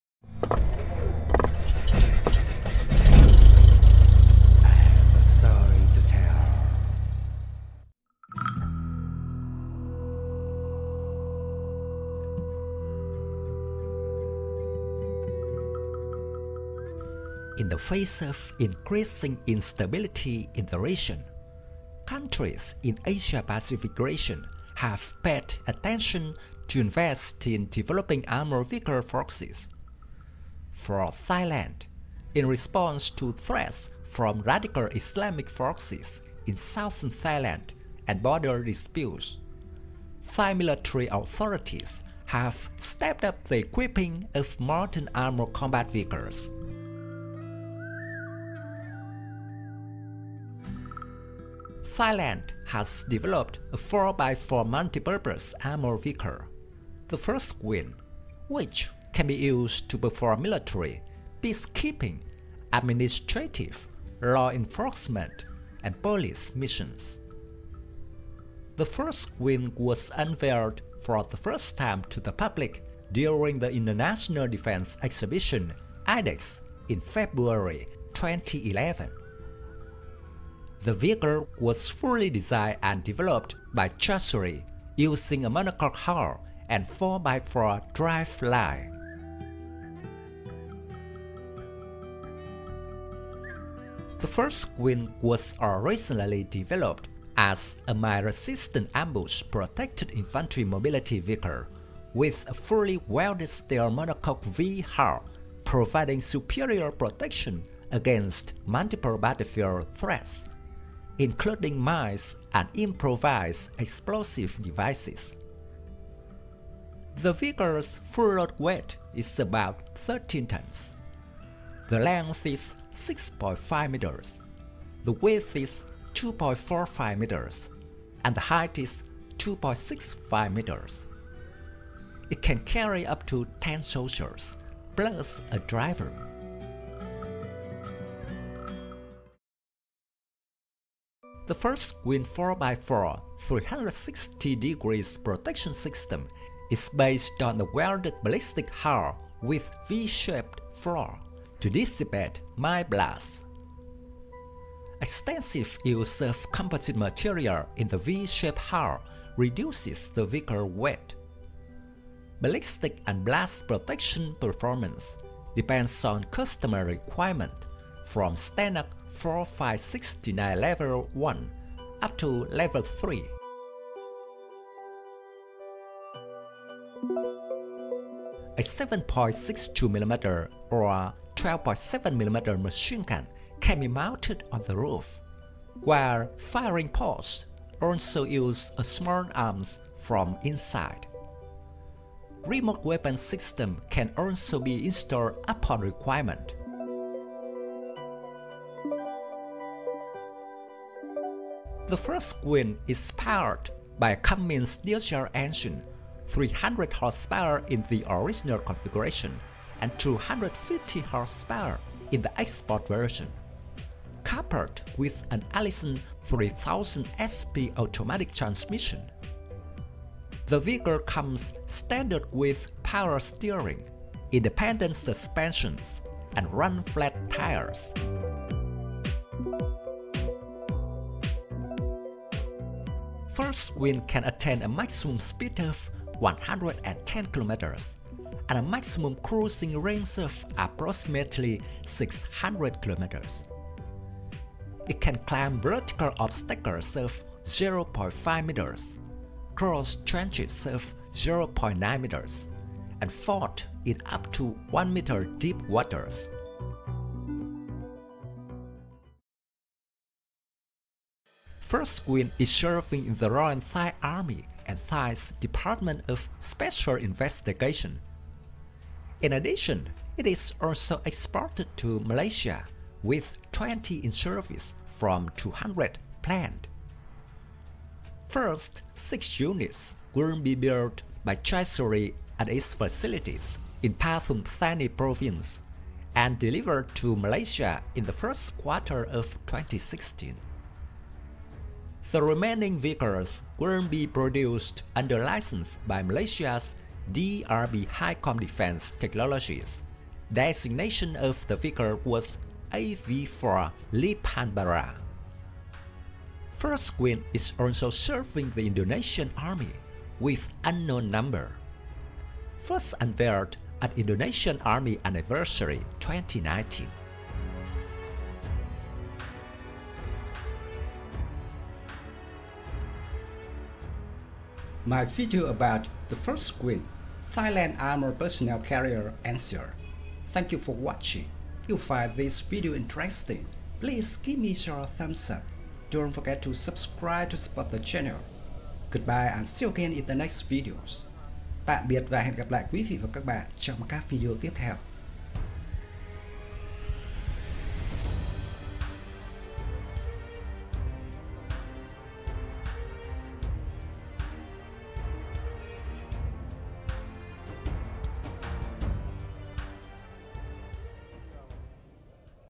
First Win: Thailand's Genuine Armoured Personnel Carrier
Thailand has developed a 4x4 multi-purpose armored vehicle, the First Win, which can be used to perform military, peacekeeping, administrative, law enforcement and police missions. The First Win was unveiled for the first time to the public during the International Defense Exhibition IDEX in February 2011. The vehicle was fully designed and developed by Chaiseri using a monocoque hull and 4x4 driveline.

Thank you very much.
Best regards
Dung Tran.